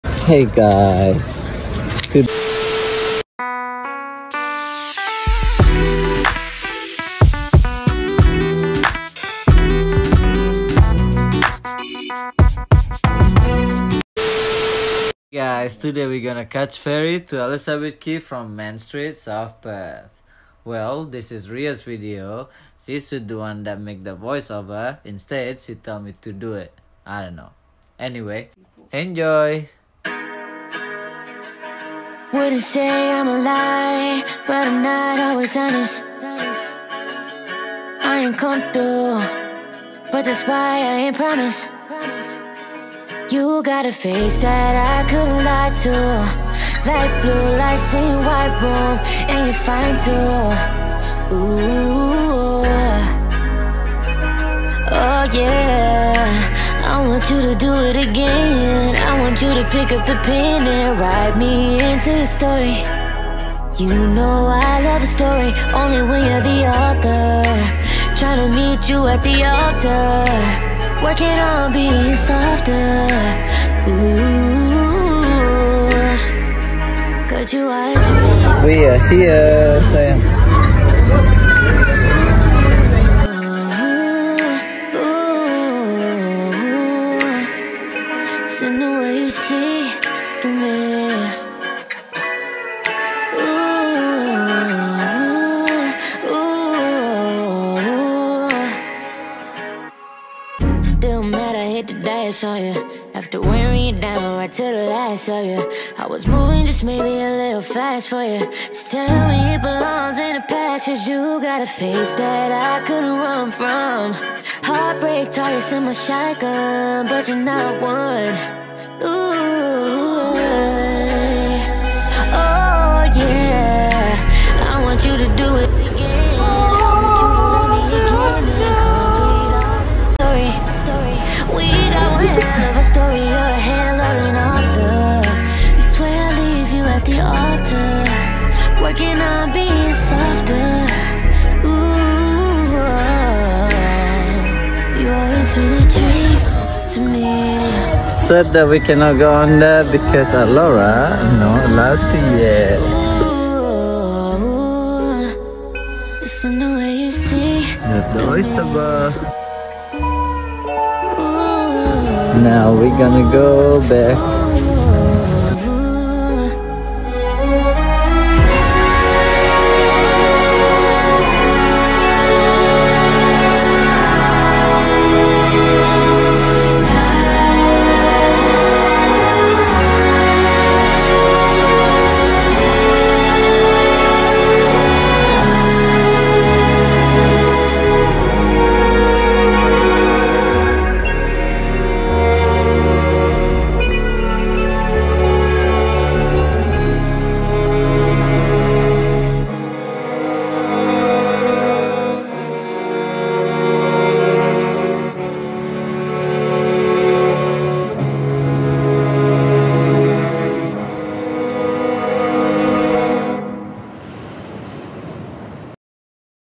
Just a short Elizabeth Quay vlog
This video is just a short vlog of me, my husband and daughter catching a ferry from South Perth to Elizabeth Quay.

Anyway I’m just your average mum that likes to try and experience new things. Remember, Be Kind, Be Humble, And always Be Grateful😊.